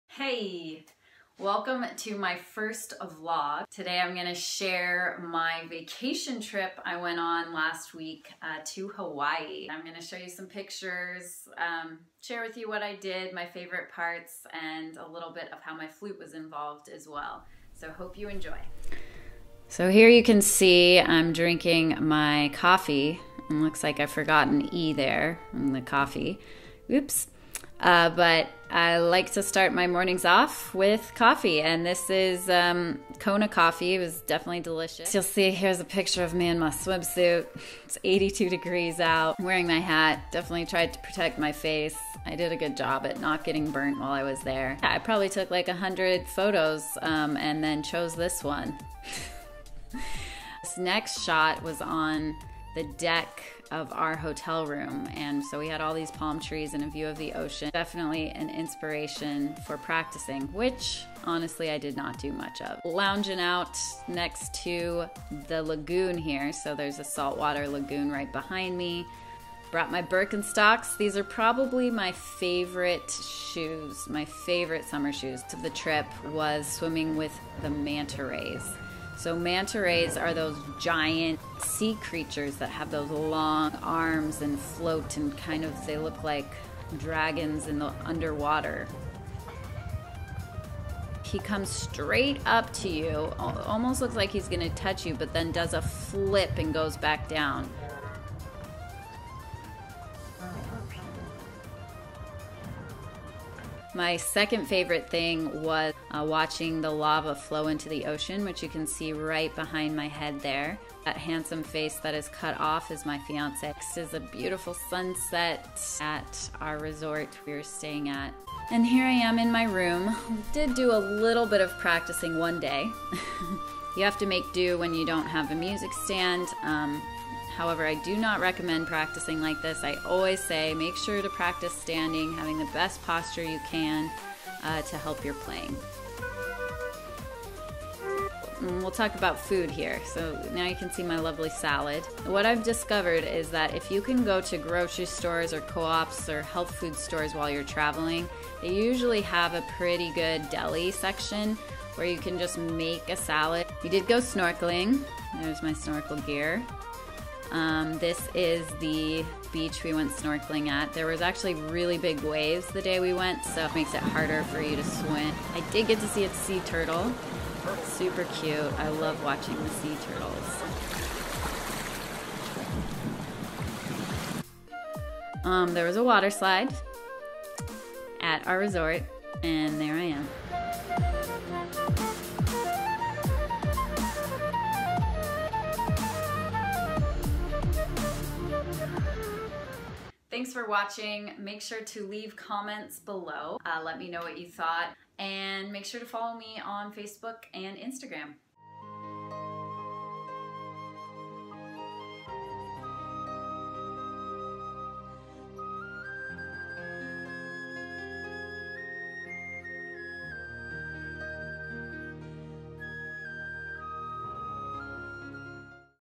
My Vacation on the Big Island of Hawaii!
Welcome to my first vlog! Hope you enjoy my pics!